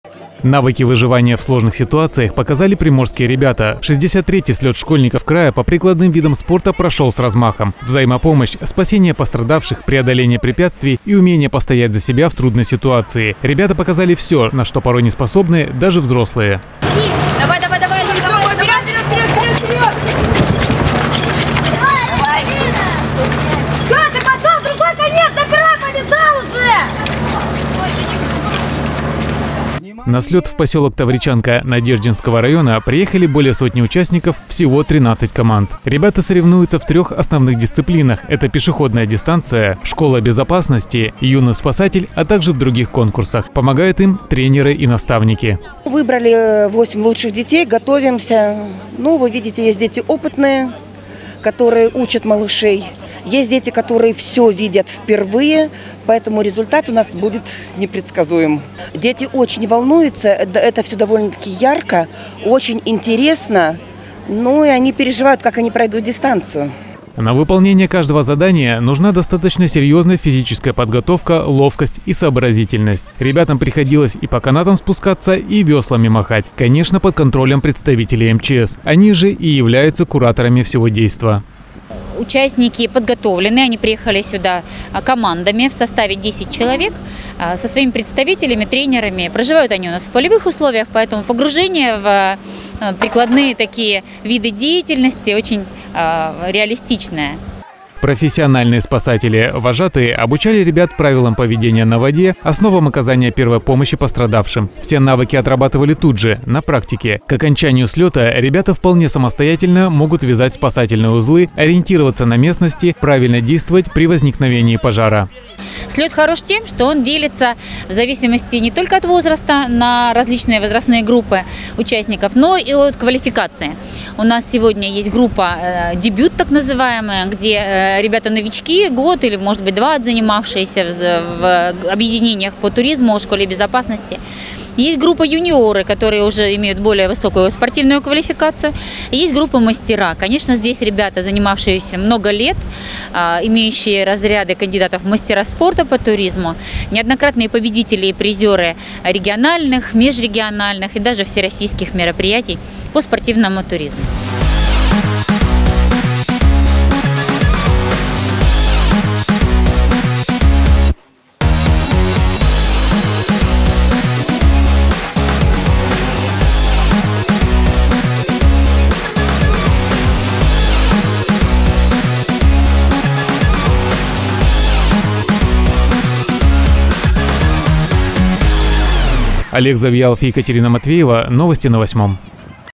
Школьниц заставили попотеть
63-й слет школьников Приморского края по прикладным видам спорта состоялся в посёлке Тавричанка. Мероприятие проходит в рамках реализации плана краевых мероприятий департамента образования и науки Приморского края и мероприятий государственной программы «Безопасный край» на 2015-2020 годы.